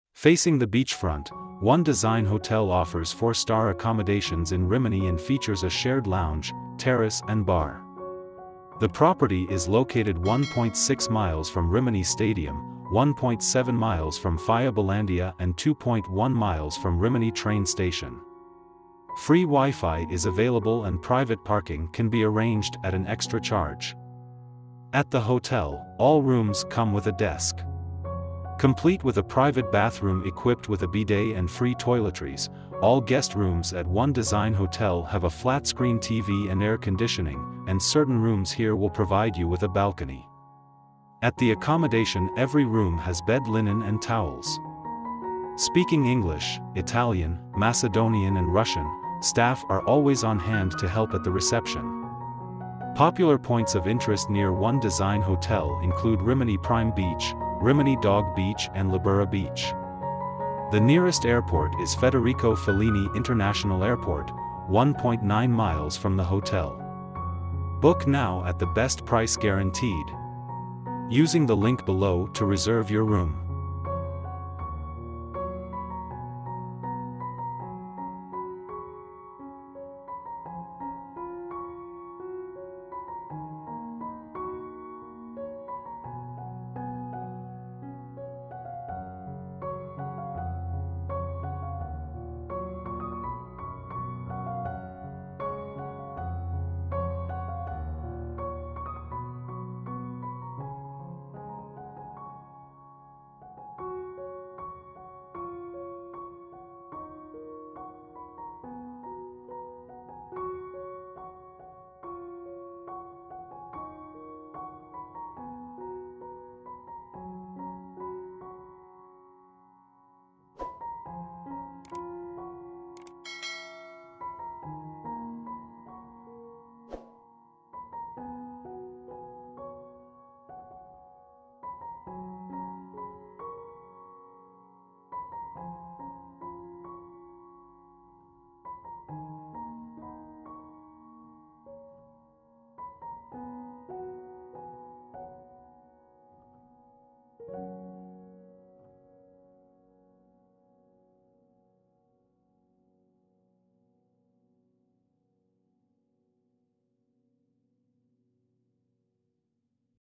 One Design Hotel
Book now

SUPPORT US
SPECIAL DISCOUNT

Facing the beachfront, One Design Hotel offers 4-star accommodations in Rimini and features a shared lounge, terrace and bar. The property is located 1.6 miles from Rimini Stadium, 1.7 miles from Fiabilandia and 2.1 miles from Rimini Train Station. Free WiFi is available and private parking can be arranged at an extra charge.

At the hotel, all rooms come with a desk. Complete with a private bathroom equipped with a bidet and free toiletries, all guest rooms at One Design Hotel have a flat-screen TV and air conditioning, and certain rooms here will provide you with a balcony. At the accommodation every room has bed linen and towels.

Speaking English, Italian, Macedonian and Russian, staff are always on hand to help at the reception.

Popular points of interest near One Design Hotel include Rimini Prime Beach, Rimini Dog Beach and Libera Beach. The nearest airport is Federico Fellini International Airport, 1.9 miles from the hotel.